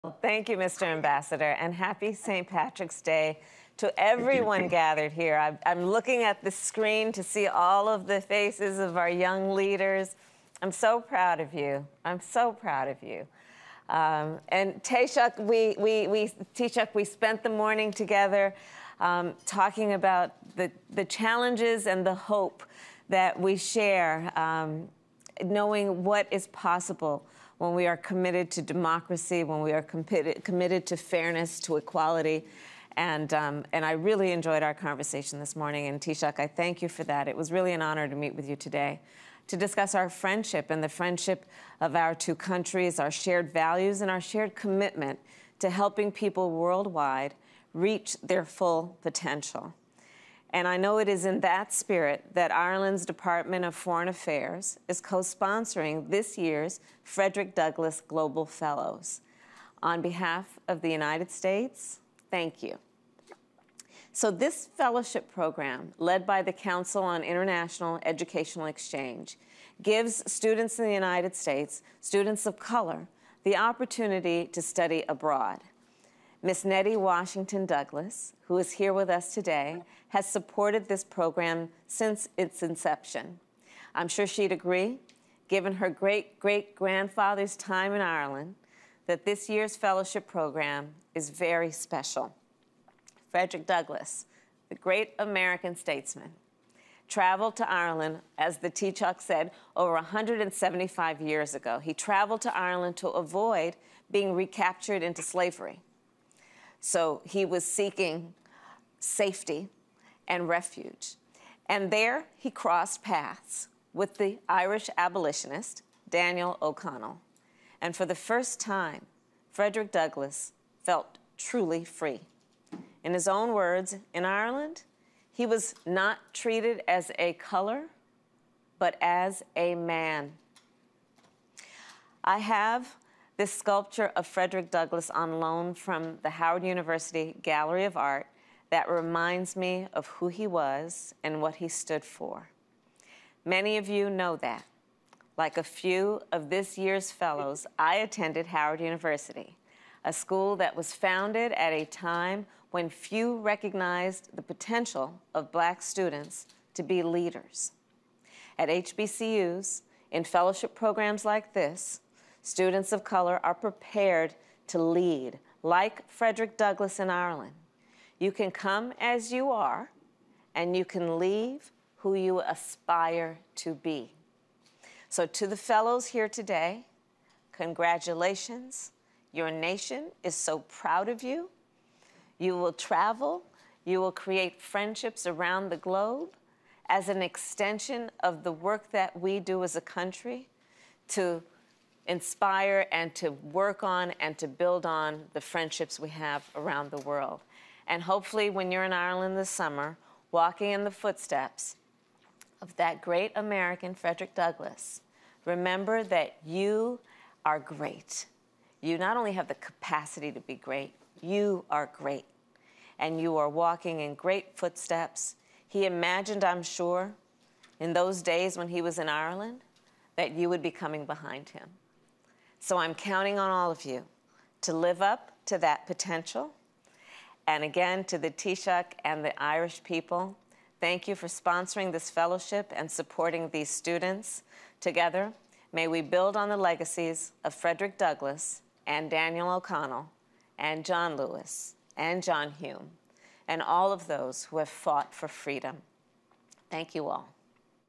VP Harris gives closing remarks to Frederick Douglass Global Fellows
Vice President Kamala Harris gives closing remarks to the Frederick Douglass Global Fellows.